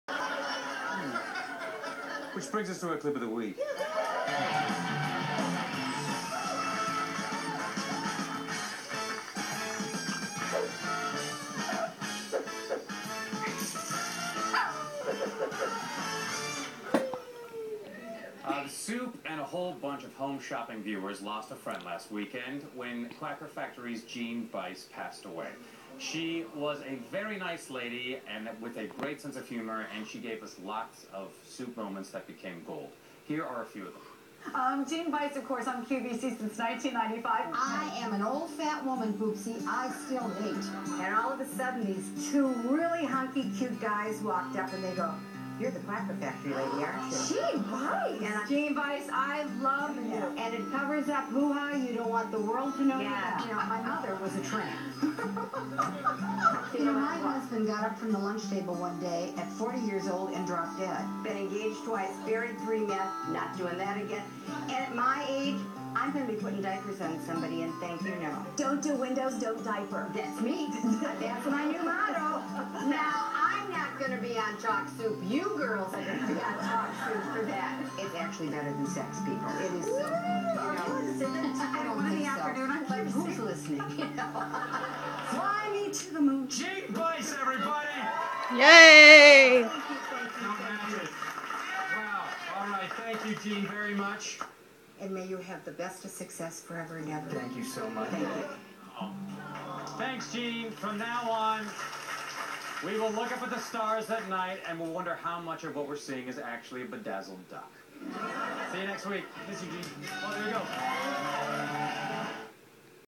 Jeanne Bice tribute
I don't own this material. I was making a video of the greatest soup moments and was shocked Jeanne Bice video wasn't on YouTube so I took it upon myself to record one. Definitely a top soup moment. May Jeanne Bice rip. love you Jeanne.